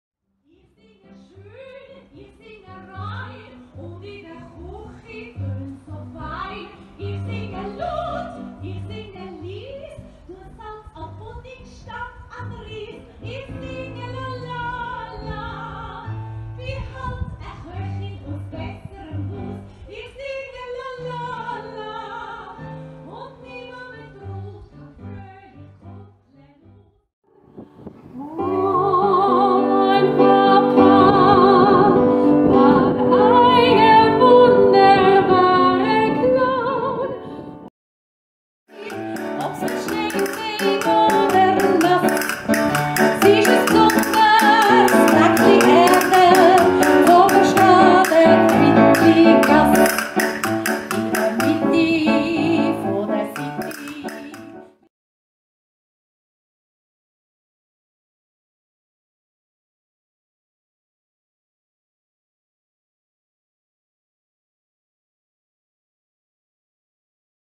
Outdoorkonzert in Kloten mit Barbara Baer & Fulvio Granata
Wir haben an 3 Standorten jeweils 20min rund um das Alters- und Pflegeheim Spitz in Kloten gesungen. Es war zwar sonnig, allerdings sehr windig. Dafür haben uns sehr herzliche BesucherInnen  dort empfangen und wir waren bestens begleitet von Jeannette Lorenzoni und Roland Keil. Vielen Dank an Alle!